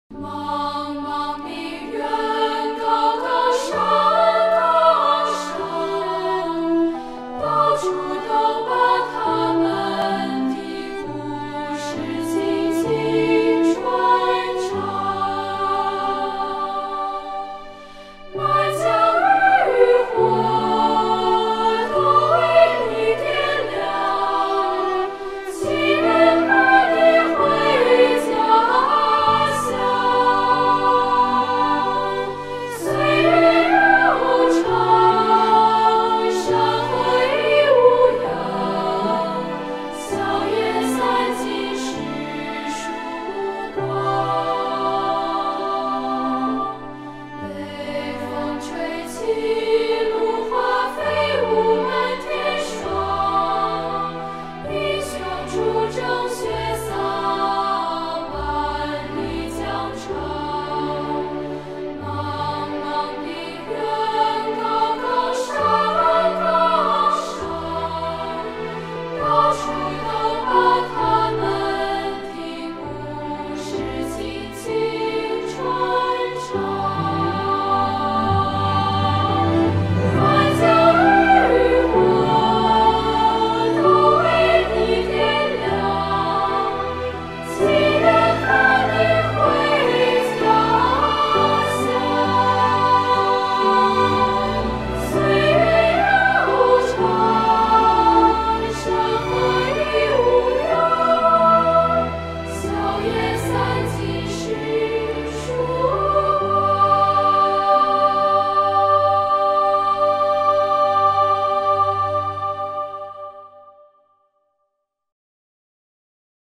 英雄回家丨120秒回顾志愿军烈士遗骸归国之路
离家还是少年身，归来已是报国躯！山河无恙，盛世如您所愿，铭记历史，缅怀先烈！